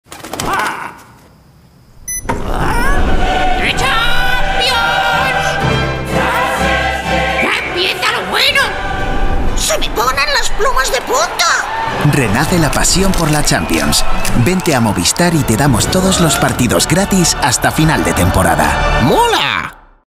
MOVISTAR: Renace la Champions
Renace la pasión por la Champions. Todos los partidos, TODOS, están en Movistar